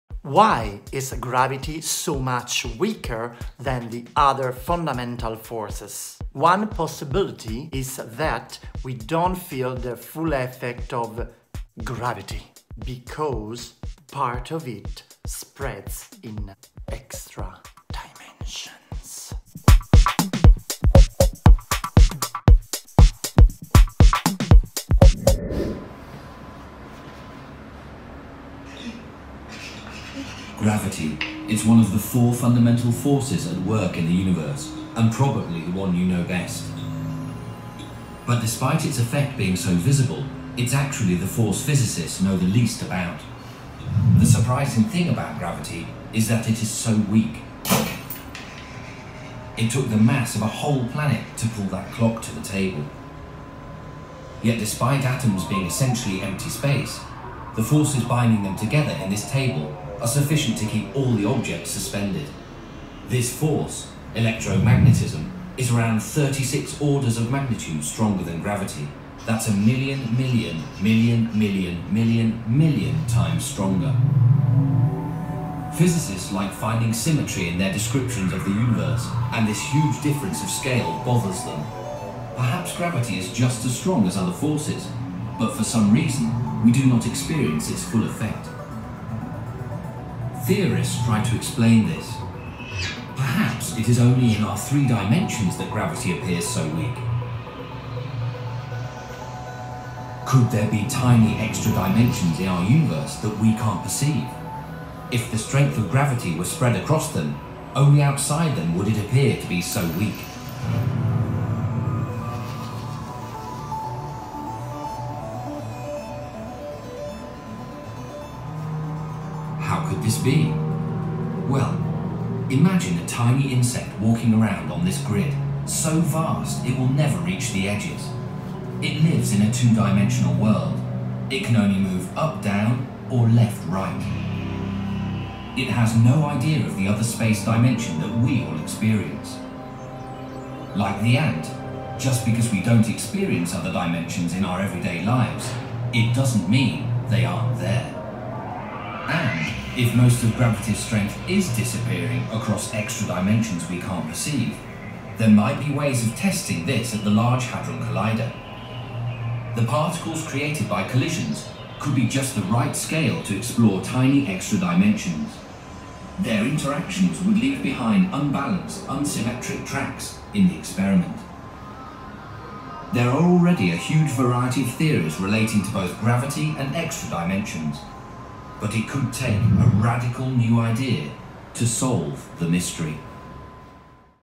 EXTRADIMENSIONS 😱😱 - the physics we like 👍🏼
❤️ 𝗡𝘂𝗼𝘃𝗼 𝗟𝗶𝗯𝗿𝗼
Why is gravity so much weaker than the other fundamental forces?One possibility is that we don’t feel the full effect of gravity, because part of it spreads to extra dimensions. Really?Maybe!!